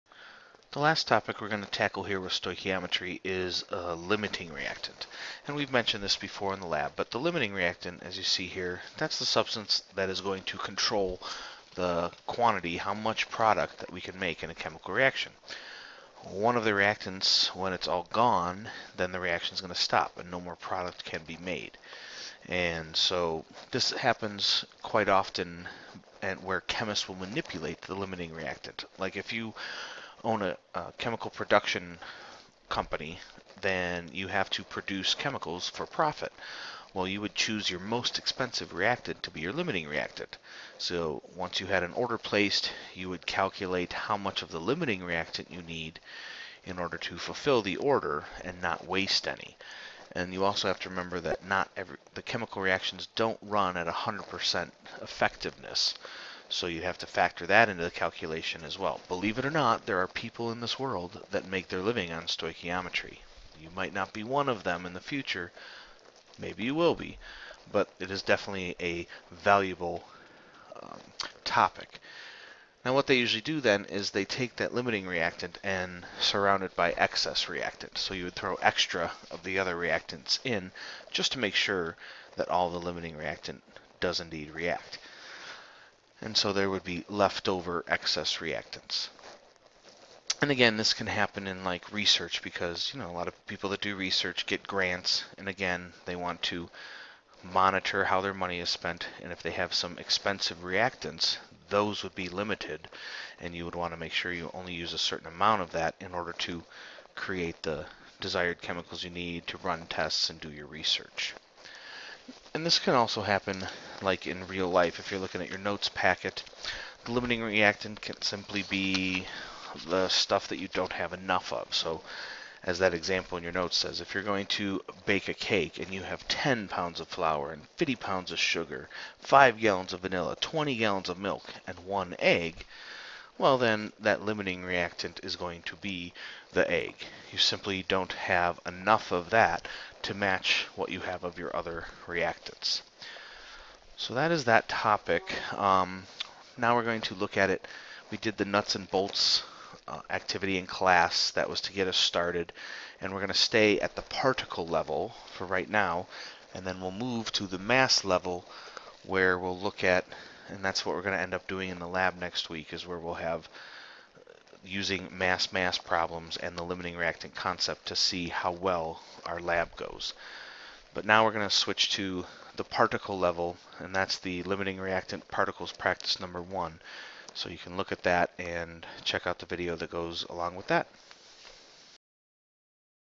Chemical Reaction Limiting Reactant Definition
Definition of a limiting reactant for my General Chemistry class.